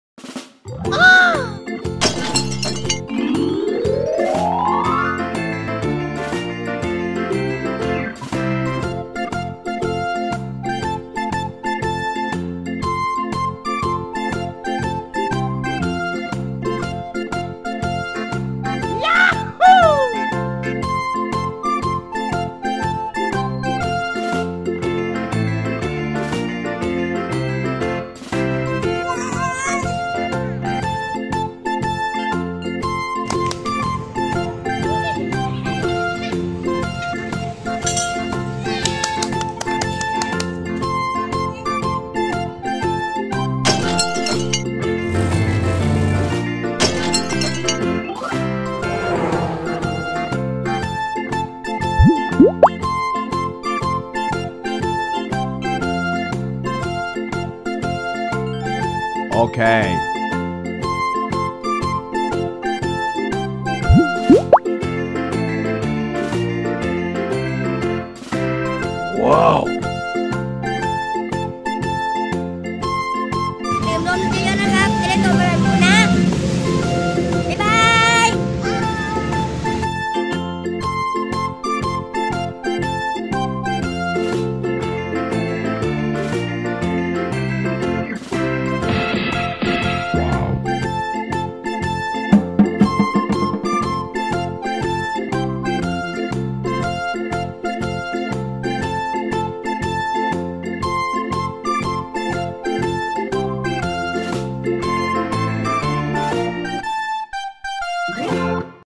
/สนามเด็กเล่นแสนสนุก prame channel
วันนี้คุณพ่อพาเด็กๆ ไปเล่นสนามเด็กเล่นกันครับ มีไต่ราว สไลด์เดอร์ ไต่เชือก โหนห่วง ไปดูกันเลยว่าเด็กๆจะสนุกกันขนาดไหน